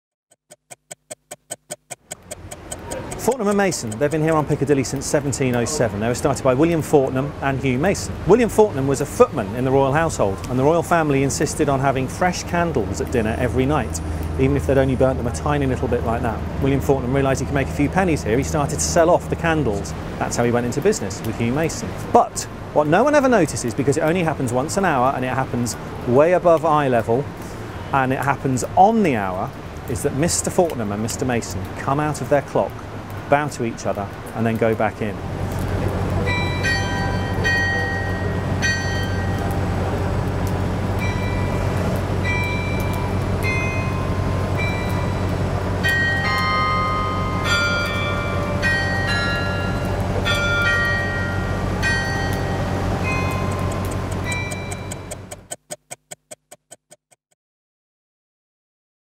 The Fortnum and Mason Clock
Mark Mason continues pointing out the small quirky things in London, this time with the clock above the Fortnum and Mason shop on Piccadilly